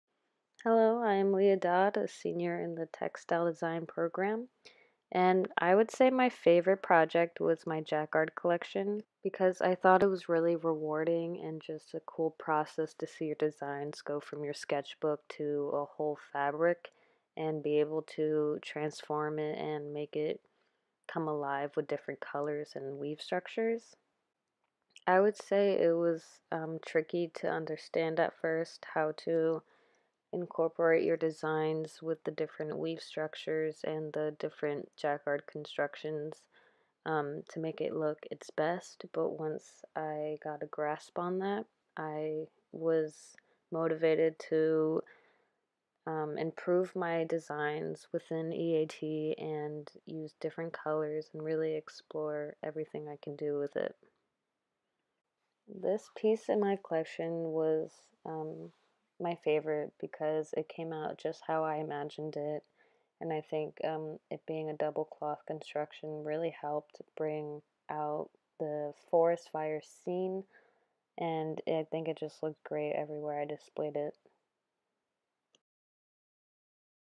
Leah Dodd - Jacquard Development
Senior Textile Design student, Leah Dodd, takes you through her Jacquard Woven Collection.
She speaks about the joy of learning to program Jacquard woven fabrics. The magic of taking a design from concept to sketch to the finished beautiful fabric and building confidence in her process.